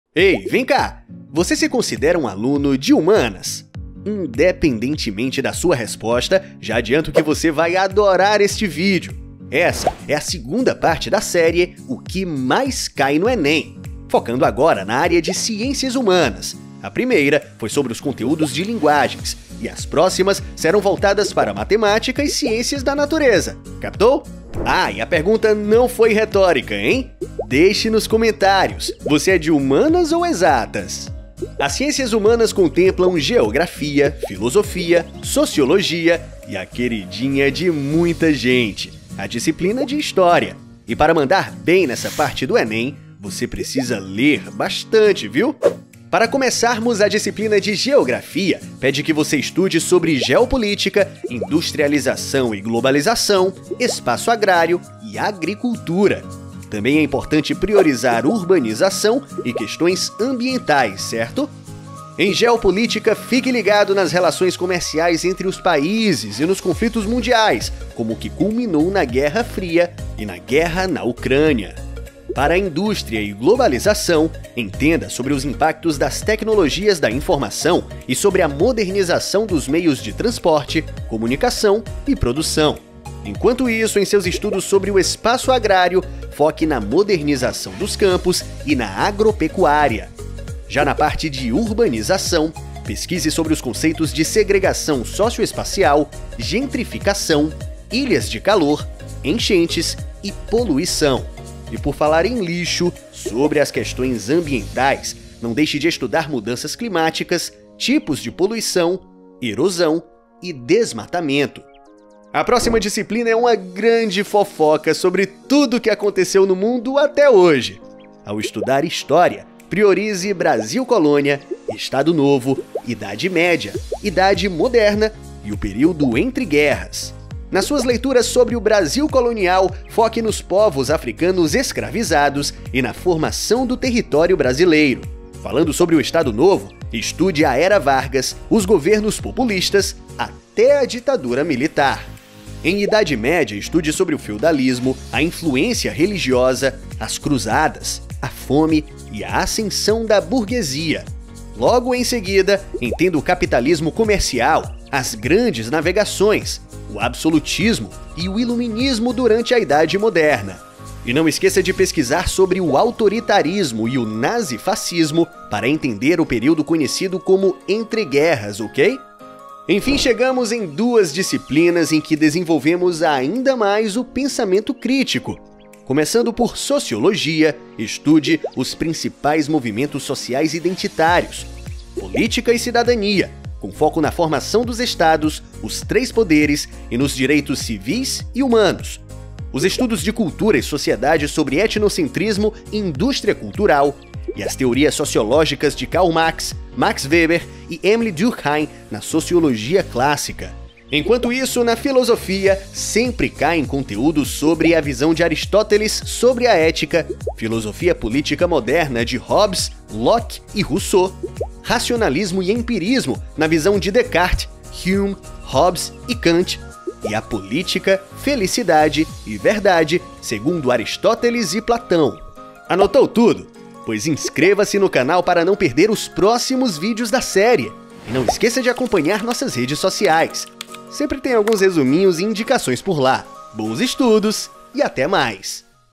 O que mais cai em Ciências Humanas no Enem? | ENEM ACTION
Está se preparando para o ENEM e quer saber quais temas são mais recorrentes na área de Ciências Humanas? Esse é o segundo de uma série de 4 vídeos sobre o que mais cai em cada área de conhecimento no Enem!
Nele, vamos te mostrar os assuntos que mais caem na prova e como estudar de maneira eficiente para garantir uma ótima pontuação!

Com essas informações, você poderá direcionar seus estudos para o que realmente importa e otimizar seu tempo de preparação. Assista agora e descubra como se preparar de maneira inteligente para Ciências Humanas no ENEM 2024!

Quer chegar super preparado para o exame e aumentar suas chances de aprovação?